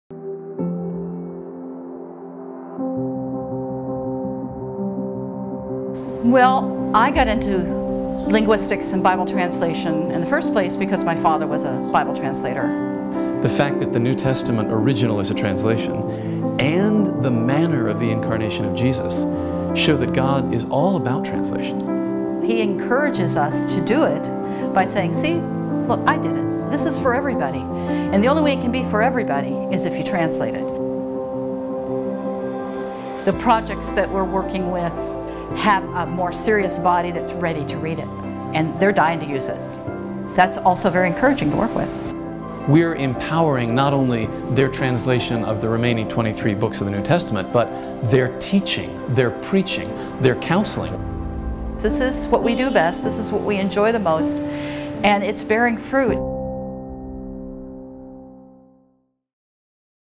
Translating the Gospel for All to Hear
“We’re empowering not only their translation of the remaining 23 books of the New Testament, but [also] their teaching, their preaching, their counseling.” Bible translation has a HUGE impact. Check it out!